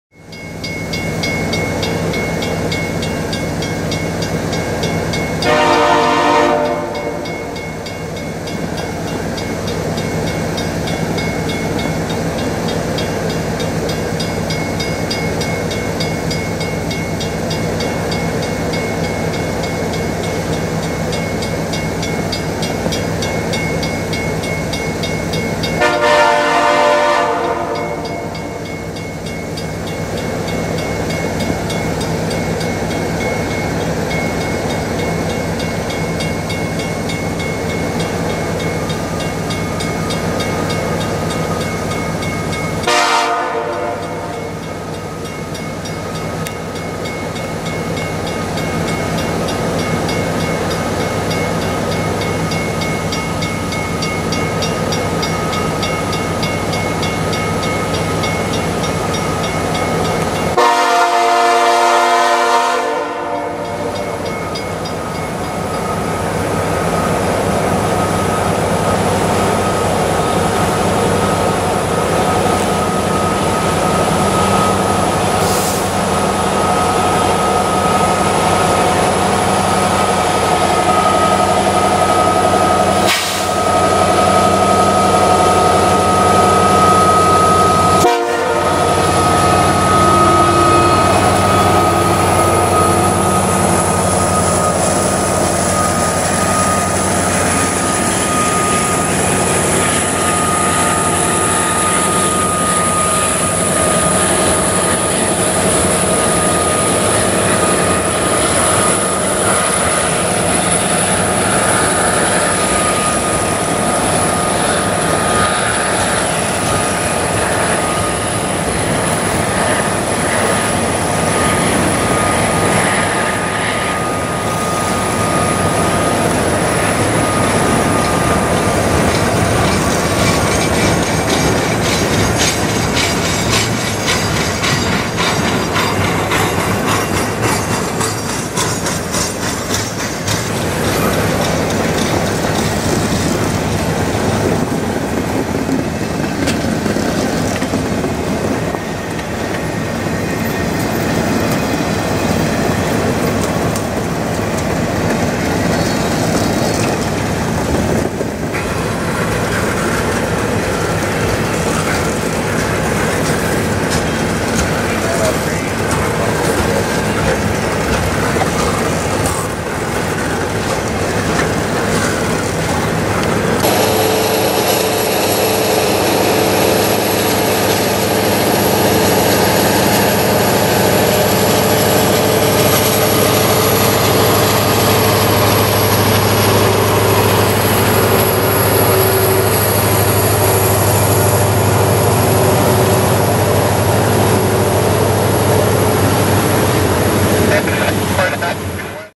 CN Train Spotting HD: CN 8903 & DPU 8943 Train Q180 With Awesome K5LLA At Carvel AB 10/10 2/7/18
Train ID: Q180
Axles: N/A
Power: CN SD70M-2 8903 SD70M-2 8943
Location: Carvel, Alberta
Milepost: 35.37
Subdivision: Edson
Extra Notes: Nice raised letter K5LLA!
Date And Time: 5:56/6:06PM 2/7/18